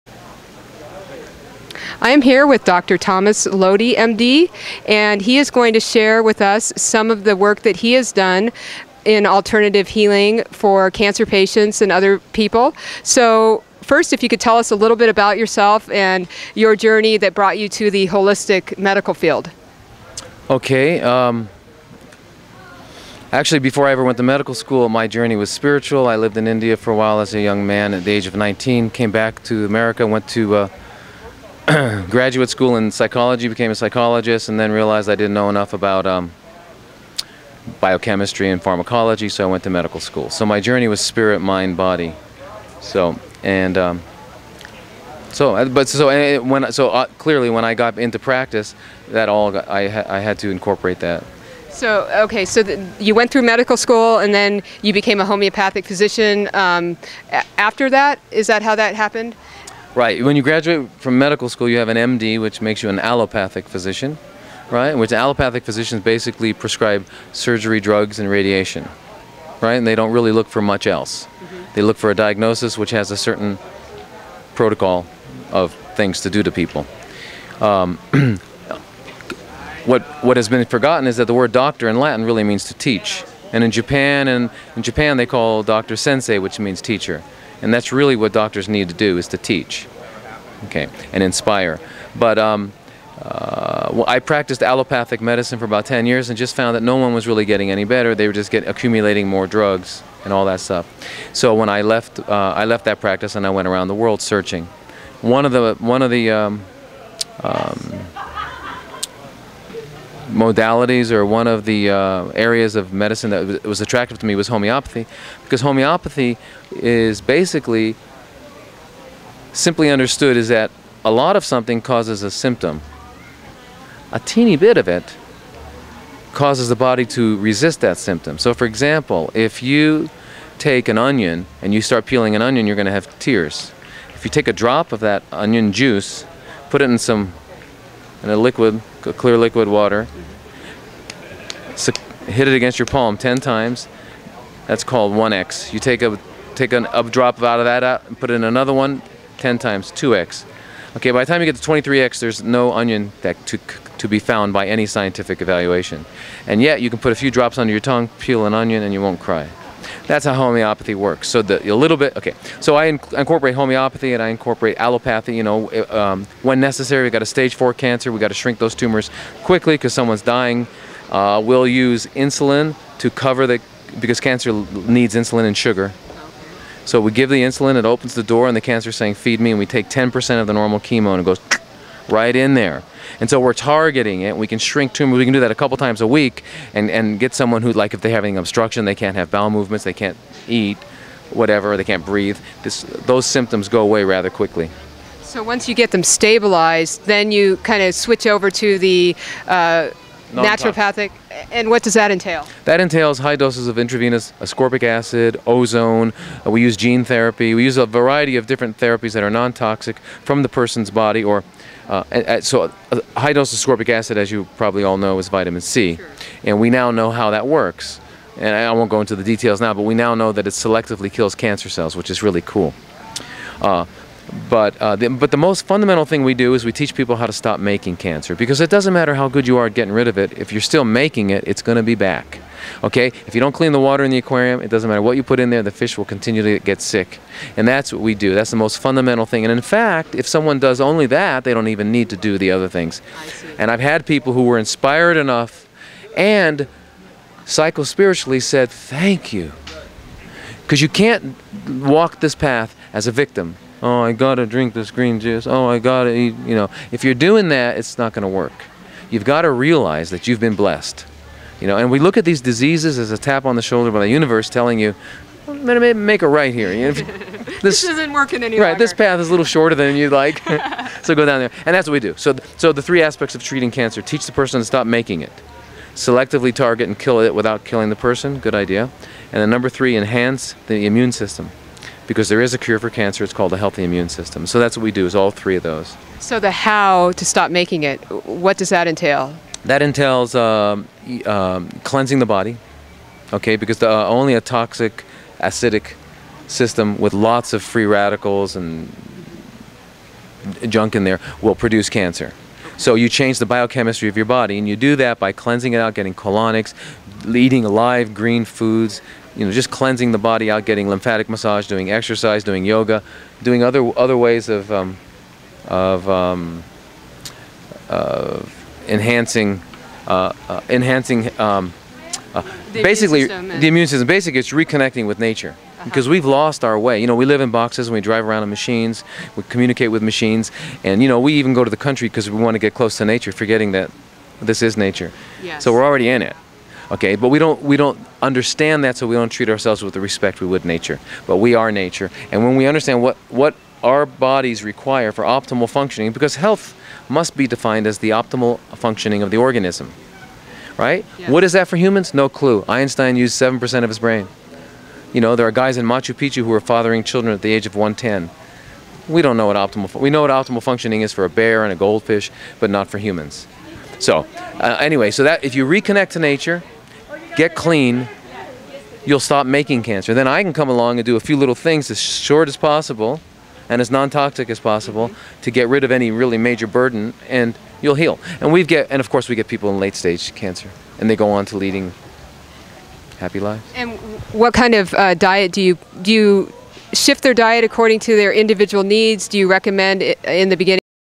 Comprehensive Cancer Care Pt. 1 - Dr Thomas Lodi
Interview with Dr. Thomas Lodi on regimen to use proper diet to mitigate various cancers.  Dr Lodi is the Founder and Director of An Oasis of Healing, and integrative cancer treatment center.